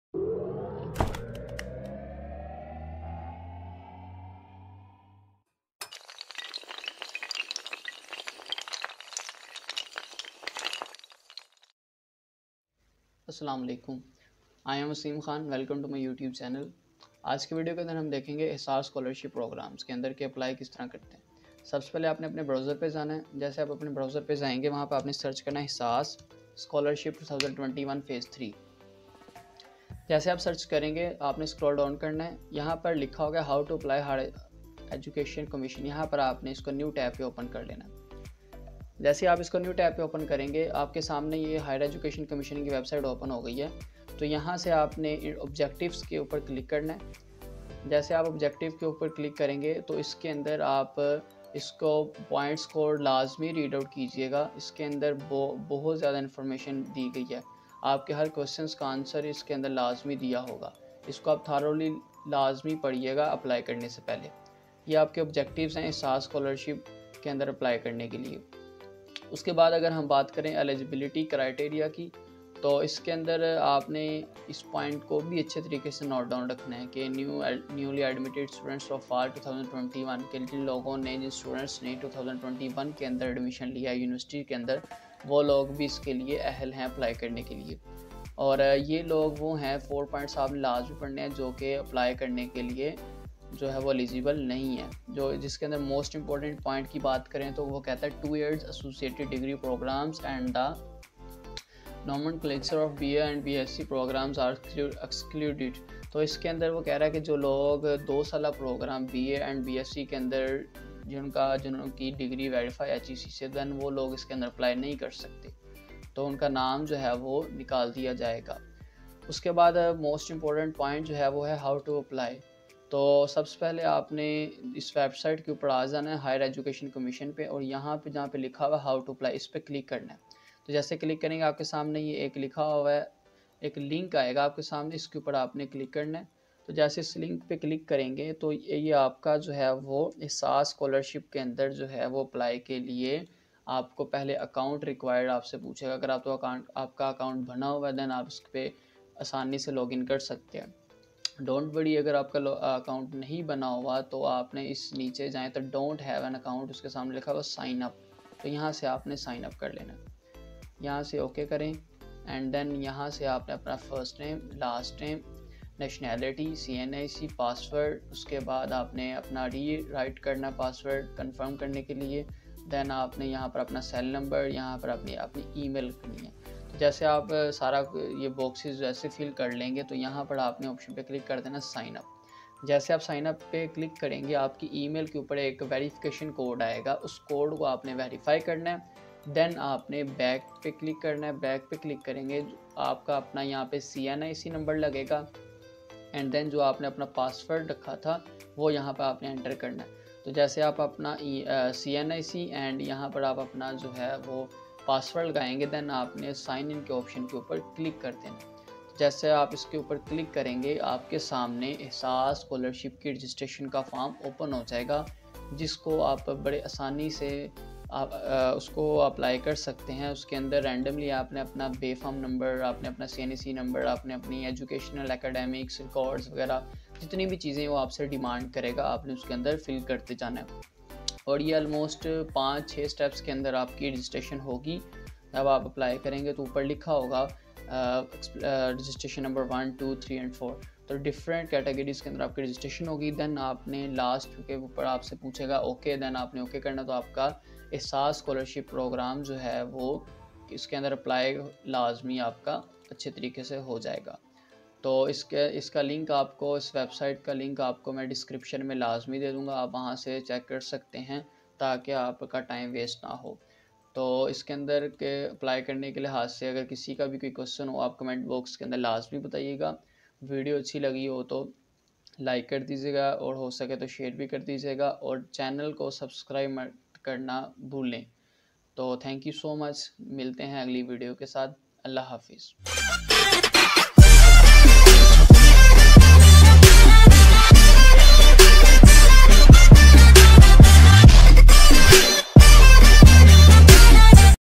HEC Ehsaas Undergraduate Scholarship Program 2021 Phase 3 Details
Online Apply

It is a paradigm shift in a policy of the Government of Pakistan to support undergraduate level students who after completing FA/FSc./Intermediate are unable to pursue university education due to limited financial resources. Unlike past financial assistance schemes that were aimed at MS or PhD programs, the Ehsas Undergraduate Scholarship Project focuses on the most critical segment, namely the 4-5year undergraduate programs, which contributes the most to income of the individual who completes the bachelors degree
It is one of the sizeable project funding under Government of Pakistan's Ehsaas social safety net program which aims to provide 50,000 undergraduate scholarships with an average budget of Rs.6 billion annually. Over a period of next four to five years these scholarships will be  increased to  around 200,000.
Scholarship covers university tuition fee and stipend throughout four-five years of degree duration. It is a significant step to ensure affordable higher education in  all major fields of education including agriculture, arts and humanities, business education, engineering, medical, physical sciences, and social sciences.
The project aims to support female education therefore 50% of scholarships are reserved for the female students in the overall Project's target.
To encourage physically challenged students, 2% of the scholarships are reserved for them.
 It is a vibrant program which will facilitate creation of the competent  workforce thus enhancing employability factors in youth.
It foresees to create drive for education among those students who have discontinued  studies due to  limited financial issues.
By targeting most vulnerable segment of young population through this initiative, poorer households would be enabled to come out of  poverty, which in a long run expected to have positive impact on individuals, their families and the community.

Scholarships are offered for:

Newly admitted students of Fall 2021 session  in HEC recognized public sector university
Students fulfilling criteria of need and provide authentic information required under the Project's policy.
Admissions secured on merit only. Merit means that the student has fulfilled the admission criteria of the institution in which he/she intends to study.
Degree programs allowed by the HEC.
Age limit set by institution's regulations, i.e. the maximum age of the eligibility for the scholarship is the same as the maximum age for applying for admission in undergraduate programs at the HEI.
Ineligibility Conditions:

A student is ineligible for the scholarship if found

Availing any other scholarship or continuous grant
Secured admission on self-sustain/ self-support/ self-finance basis, or in evening programs with higher fee structures
Two years associate degree programs and the nomenclature of BA/BSc programs are excluded
After submission  of the online application form of Ehsaas Undergraduate Scholarship Project , the applicant is required to take printout of the application form and attach following supporting documents:

Sr.no Supporting Documents
1 Copy of applicant CNIC / B. Form
2 Copy of CNIC (Father, Mother/ Guardian)
3 Salary Slip / Income Certificate (Father/Guardian, Mother)
4

If father is Labourer on daily wage / shopkeeper / private job, then attach undertaking of Income Certificate on minimum amount of stamp paper of Rs. 50/-
5 Copies of last Six Month Utility Bills (Electricity, Gas, Telephone, Water) – (If applicable)
6 Copy of Rent agreement in case of Rented House
7 Copies of Last Fee Receipts of Applicant and Siblings (If applicable)
8 Copies of Medical bills/ expenditure related documents (if applicable)
9 01 Passport size Photograph of Applicant
10 01 Picture of House from outside Front
Important Note: The hard copy of the application form along with the supporting documents will be submitted to the Student Financial Aid Office of the university only.